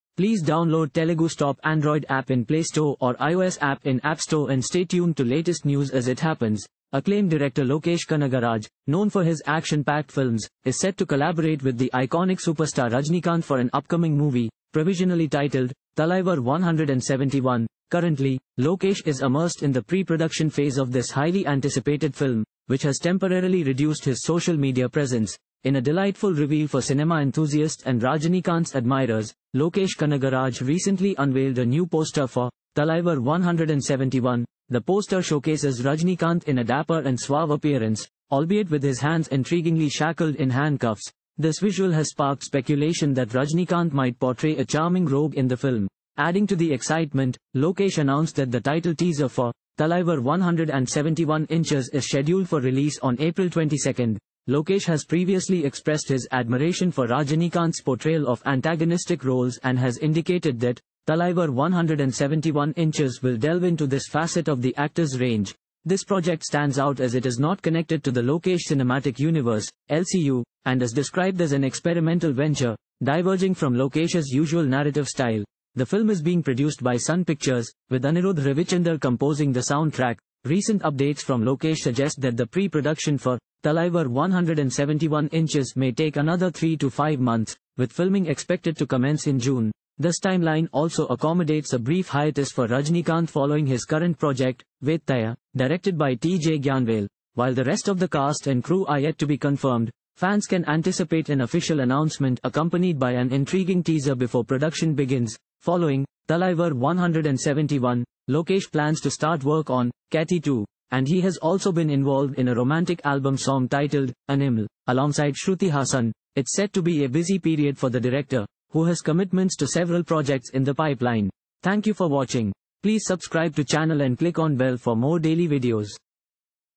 Countdown Begins: Title Of Rajinikanth & Lokesh Kanagaraj's Film Set To Drop! | Movie
Acclaimed director Lokesh Kanagaraj, known for his action-packed films, is set to collaborate with t - Lokesh Kanagaraj Countdown Begins: Title Of Rajinikanth & Lokesh Kanagaraj's Film Set To Drop!